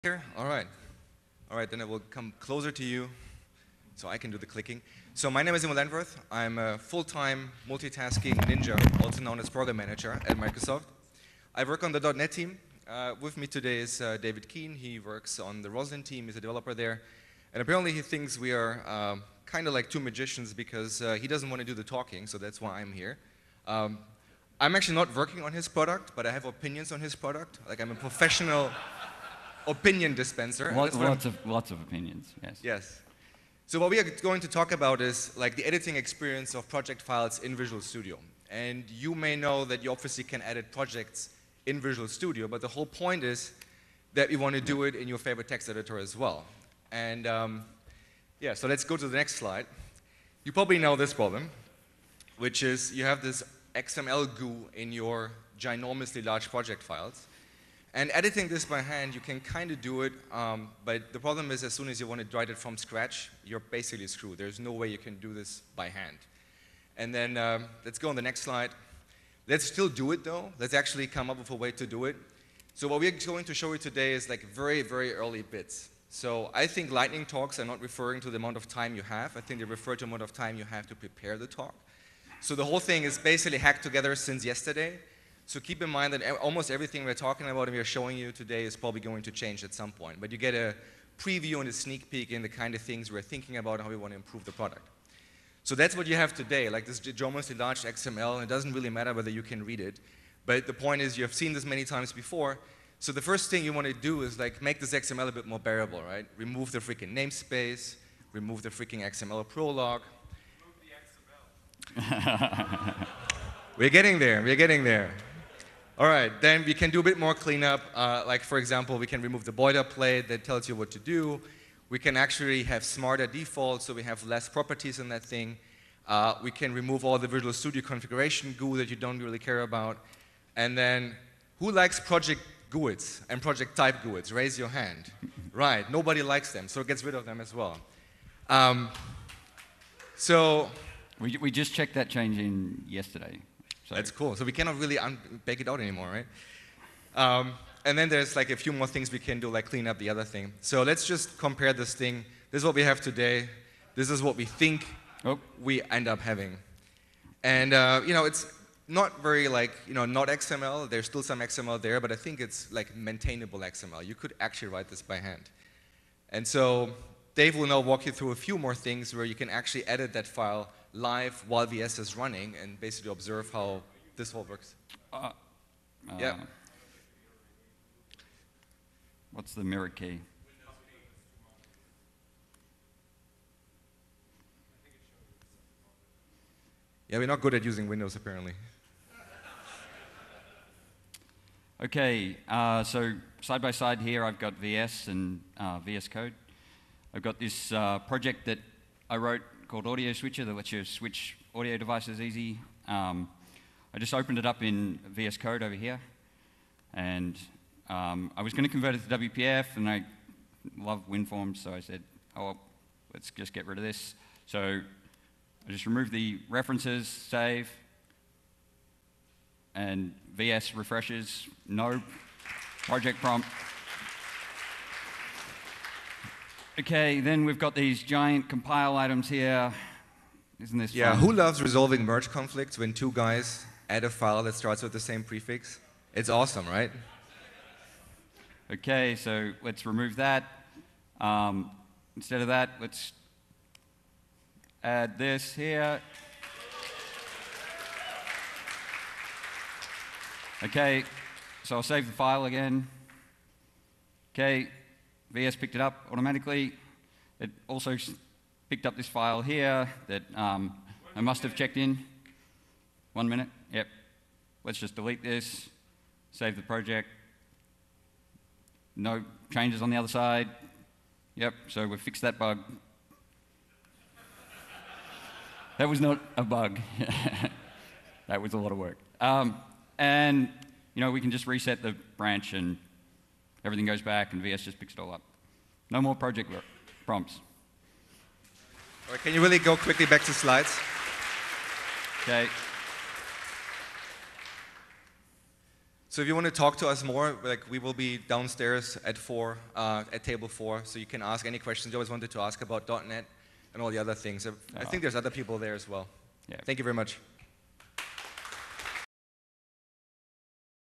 Lightning Talk - New C#/VB Project System
David Kean and Immo Landwerth
A (very) early look at some of the changes we're making to the new C#/VB project system that will get Visual Studio out of the way of the developer.